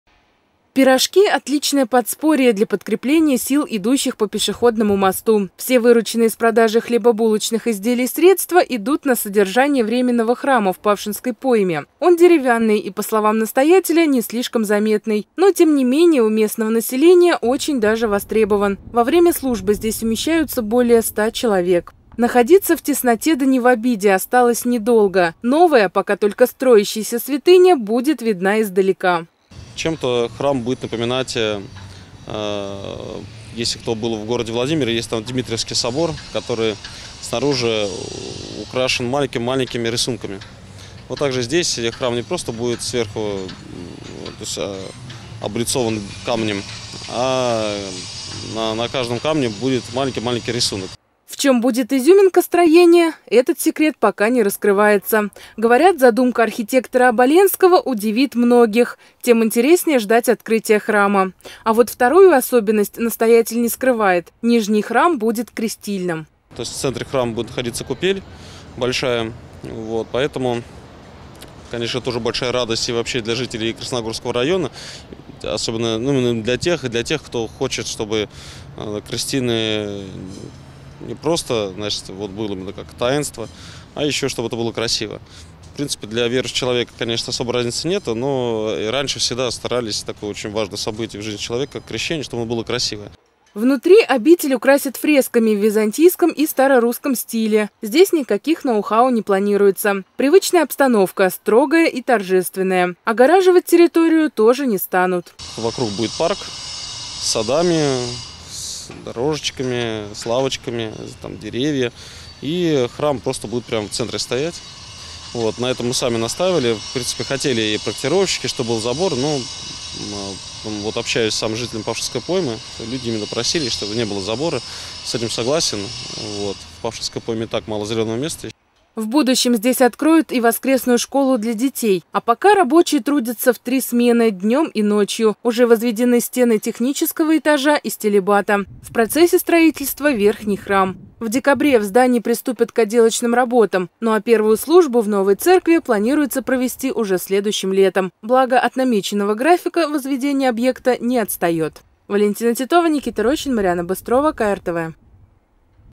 КРТВ. Никольский храм в Павшинской пойме построят в срок
Первая служба в основном здании храма в Павшинской пойме состоится уже через год.  Сейчас идет активное строительство. Облик церкви обещает быть впечатляющим – архитектором Никольского храма стал зодчий Андрей Оболенский. Как живет приход в ожидании основного здания и как продвигается стройка, выясняла Валентина Титова.